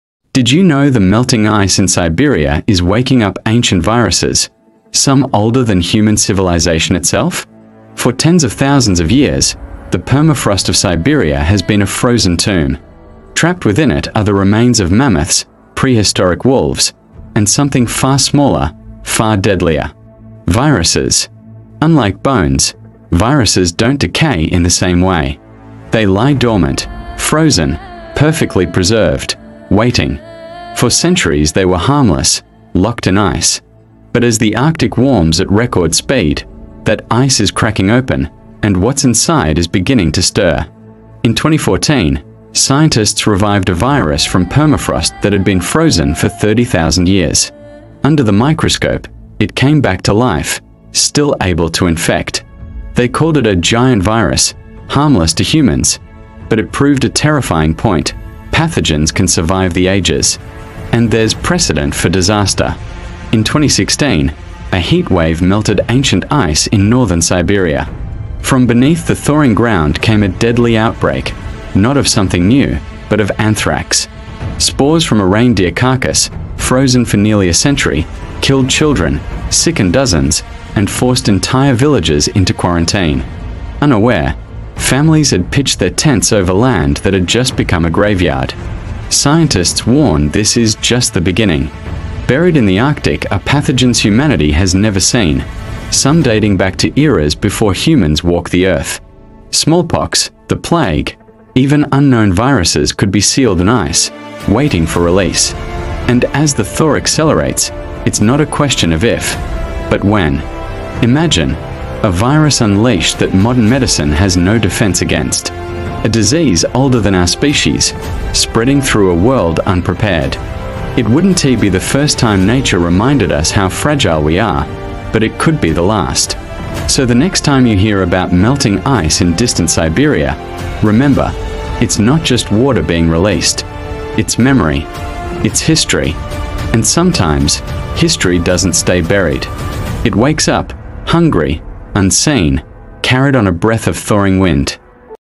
The Silent Threat in the Ice: Are Ancient Pathogens Waking Up?
Siberian permafrost is thawing at an alarming rate, and what it's revealing has scientists on high alert. In this video, we explore the discovery of ancient "zombie viruses," some frozen for over 48,000 years, that are now being revived by climate change. Are these prehistoric pathogens a danger to modern humanity?

We break down the real science behind the headlines. Learn about Pithovirus sibericum and Pandoravirus yedoma, giant viruses that have been brought back to life in the lab. Understand why these specific discoveries are a crucial warning sign for what might be locked away in the ice.